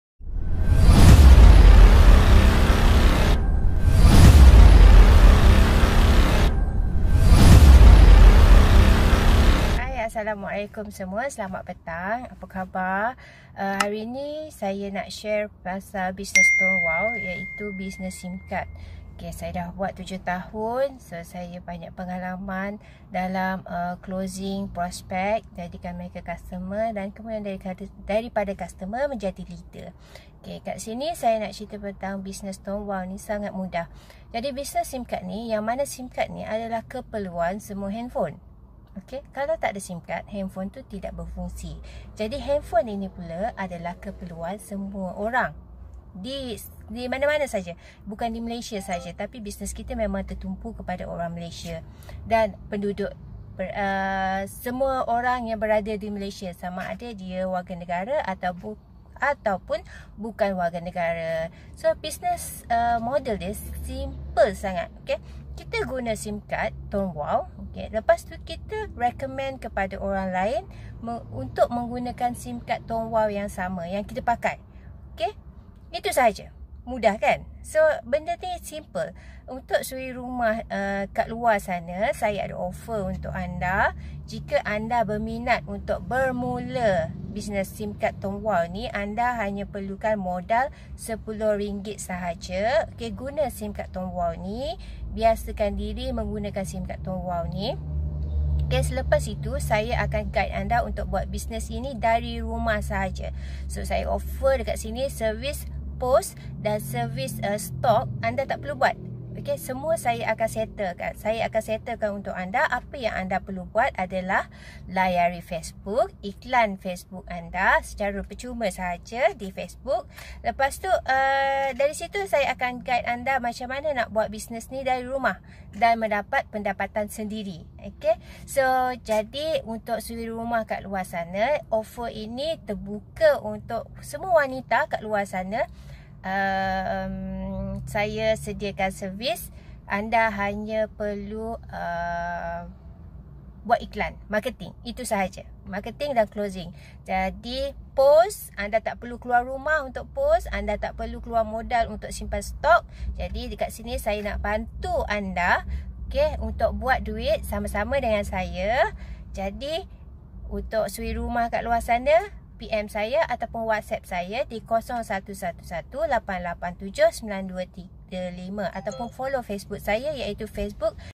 Tone Wow- Surirumah Buat Duit Ribu Ribu Dari Rumah
Buat wanita di luar sana, yang mahu buat duit tapi takde modal. Takde kenderaan, takde sijil

Saya akan tunjukkan cara buat duit dari rumah ribuan ringgit, tanpa keluar dari rumah, tanpa modal, dan tanpa simpan stok...